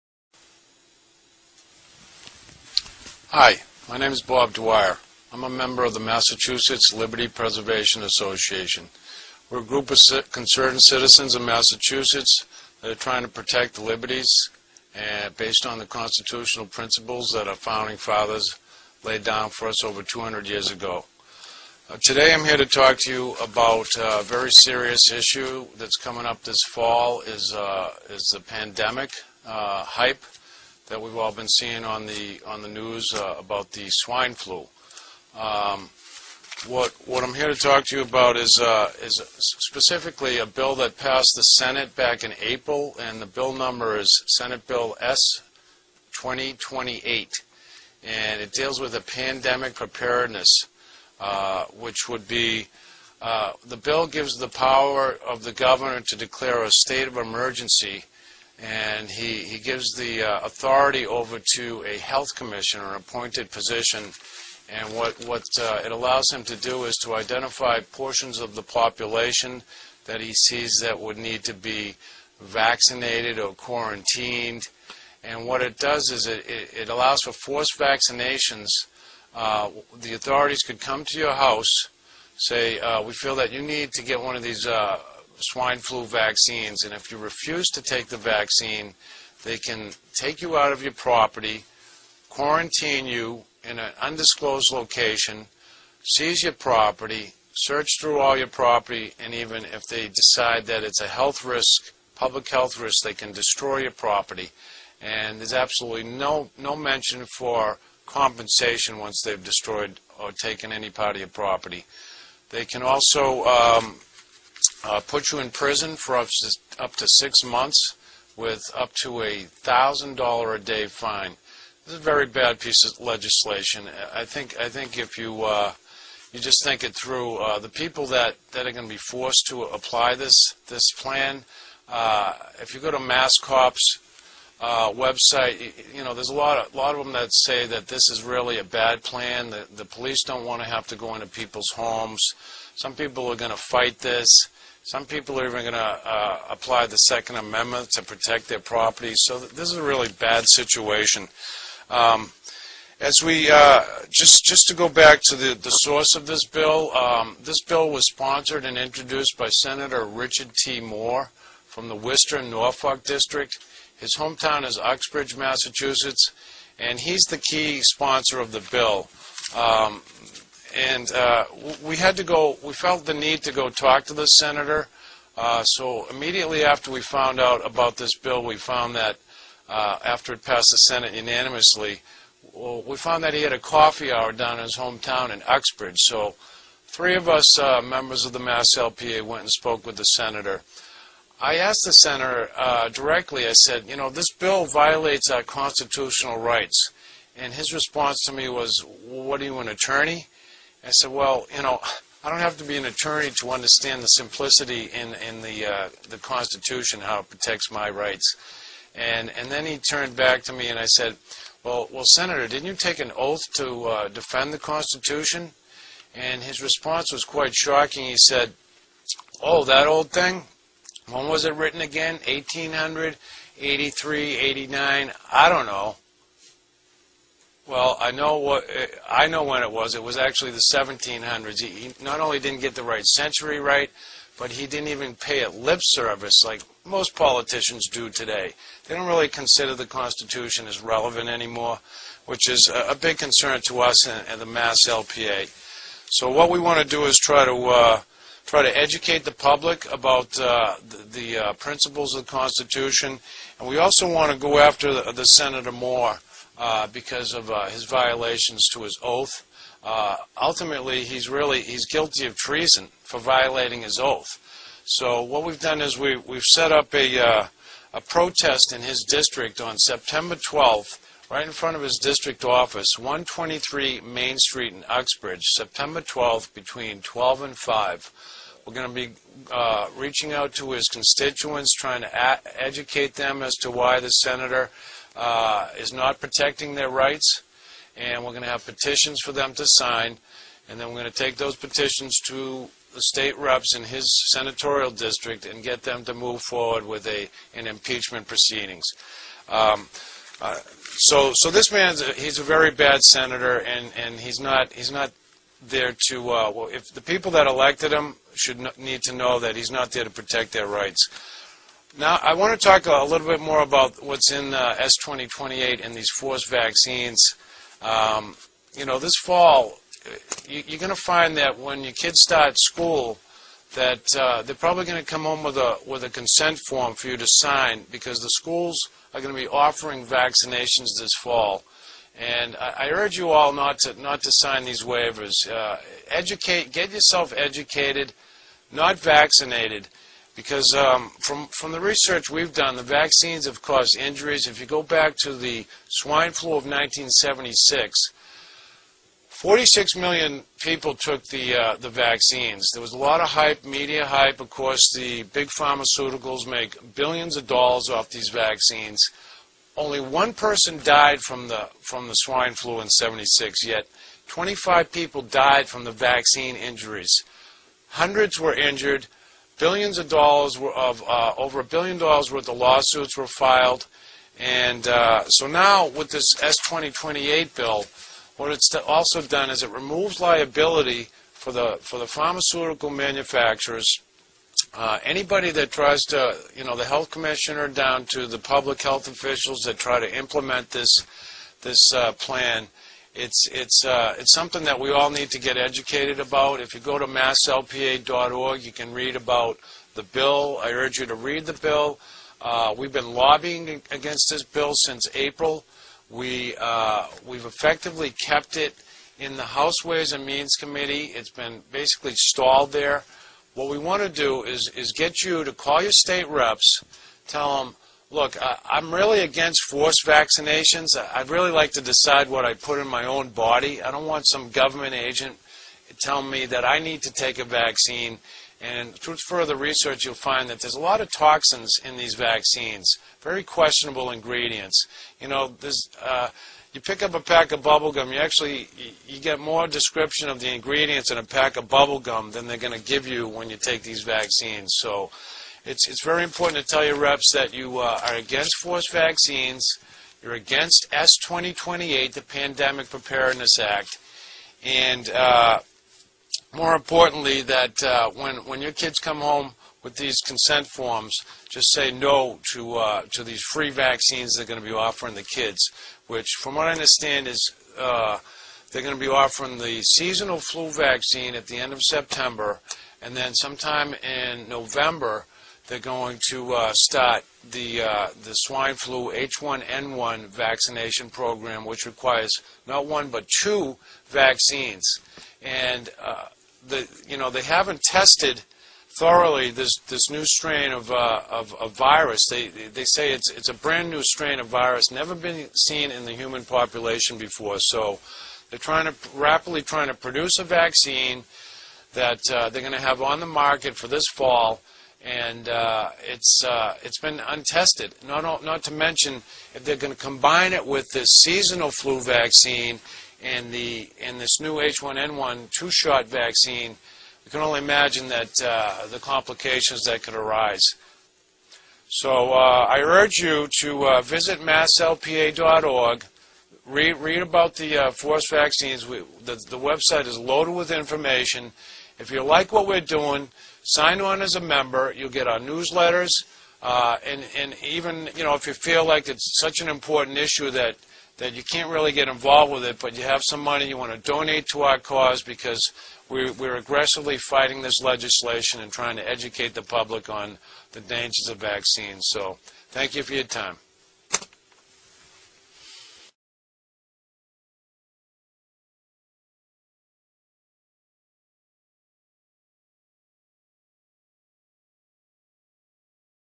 BREAKING NEWS!!!! PANDEMIC BILL PASSES- S.2028
The media is ignoring S. 2028, the flu pandemic bill that was unanimously passed by the Massachusetts Senate TODAY.
It will allow authorities to forcefully quarantine citizens in the event of a health emergency, compel health
providers to vaccinate citizens, authorize forceful entry into private dwellings and destruction of citizen
property and impose fines on citizens for noncompliance.

If citizens refuse to comply with isolation or quarantine orders in the event of a health emergency, they
may be imprisoned for up to 30 days and fined $1,000 per day that the violation continues.

MASSLPA.org comes against Senator Richard T. Moore for his stand behind the swine flu vacccines.